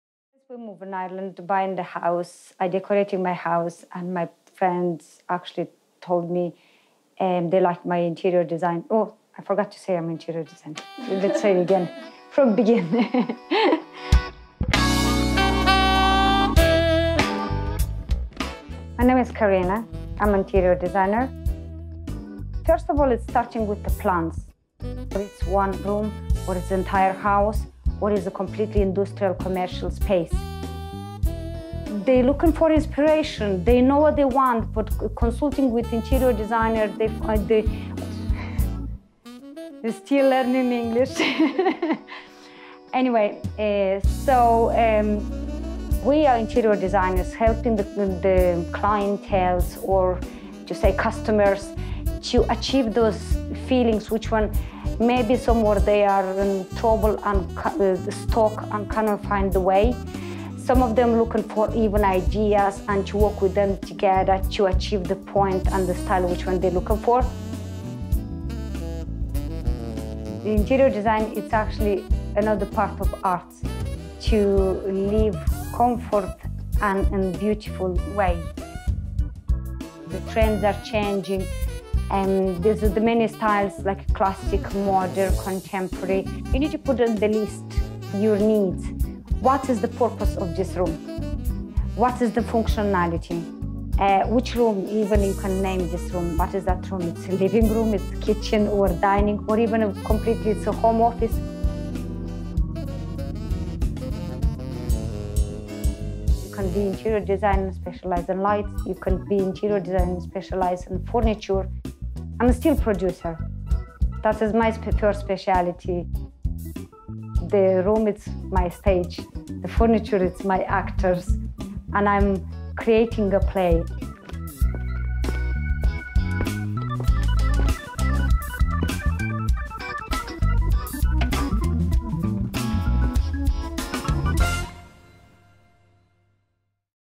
A Day In The Life of an Interior Designer
This short documentary brings us through a day in the life of an interior designer living in Ireland.